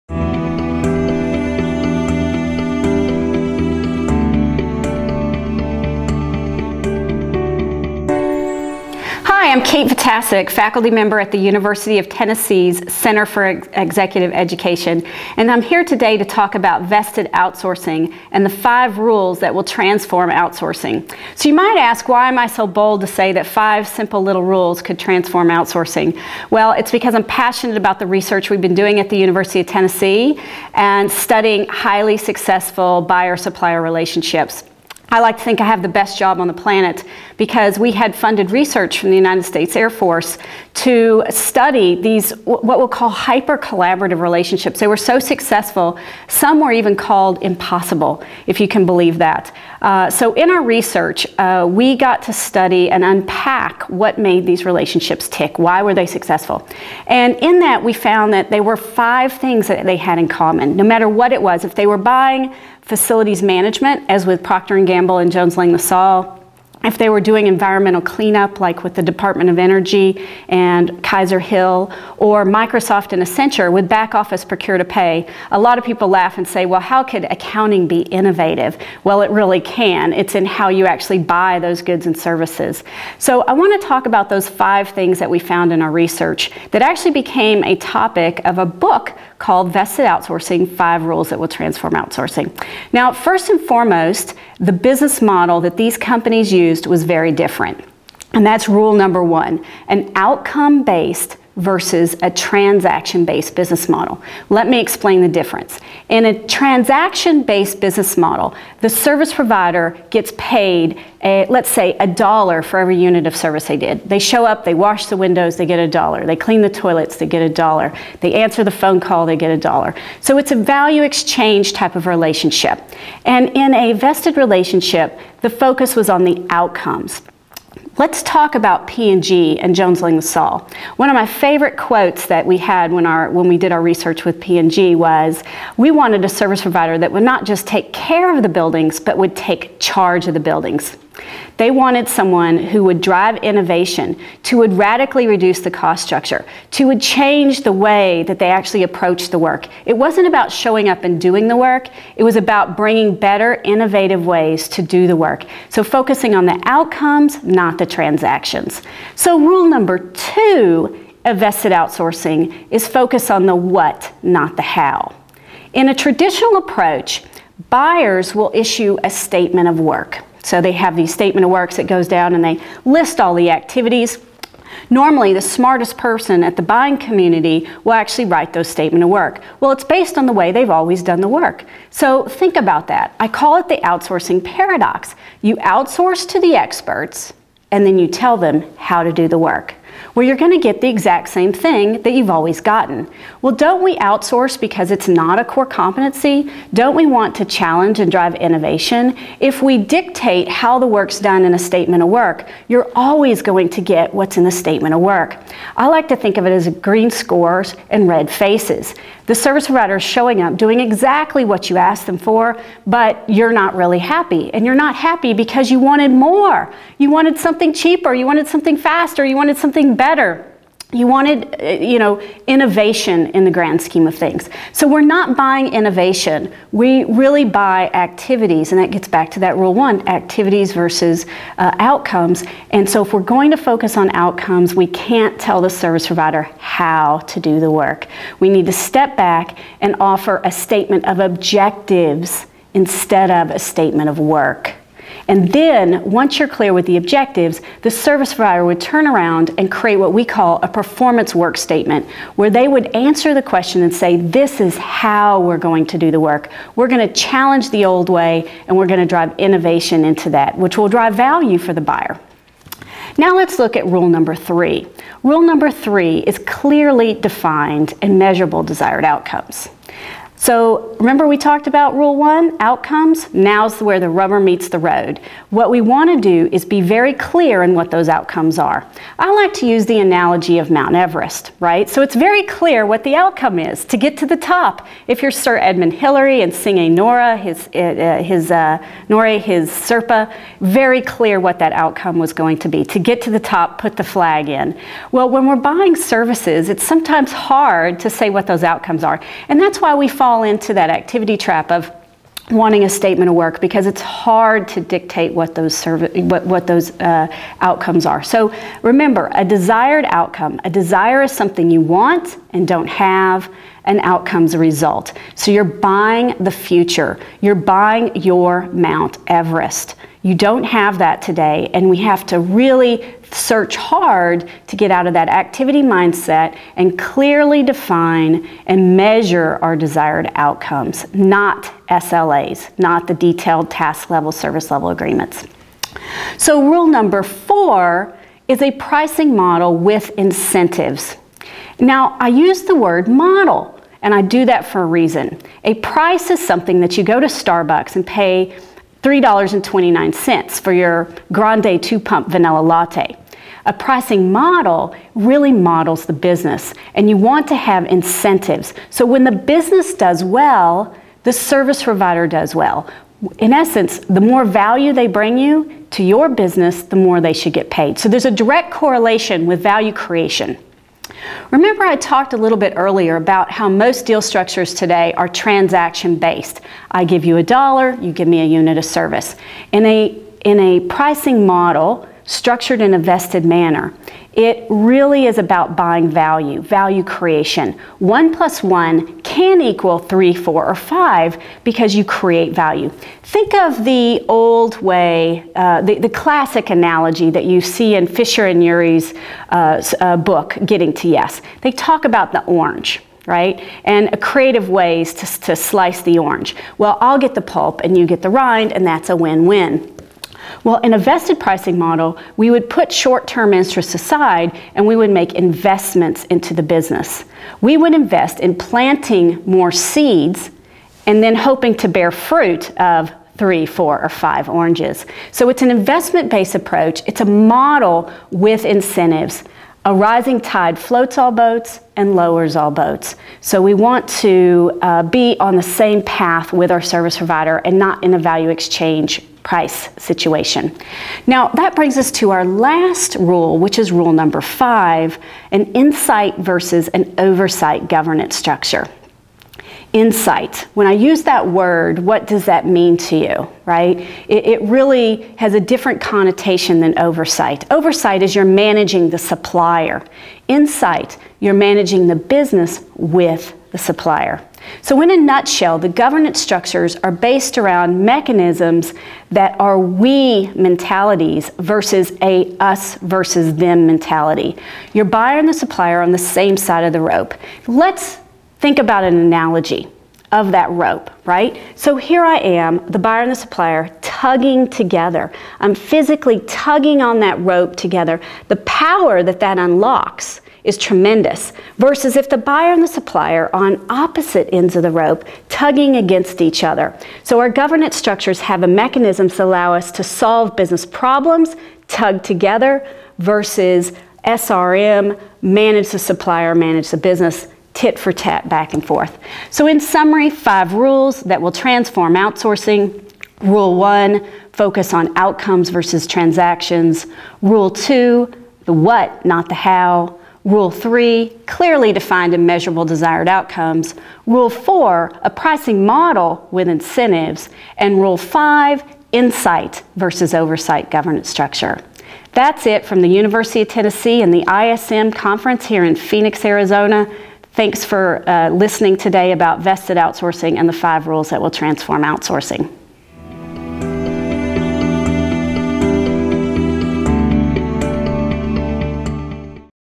Vested Outsourcing: Five Rules That Will Transform Outsourcing
In her book, "Vested Outsourcing: Five Rules That Will Transform Outsourcing," Kate Vitasek talks about three case studies, with P&G, McDonalds and Microsoft, and their successful supplier relationships.  A common theme found in her study was the shift away from a traditional customer-supplier relationship to a highly collaborative relationship aimed at developing a comprehensive, win-win solution.

Recorded at the recent ISM Services Conference (Phoenix, AZ)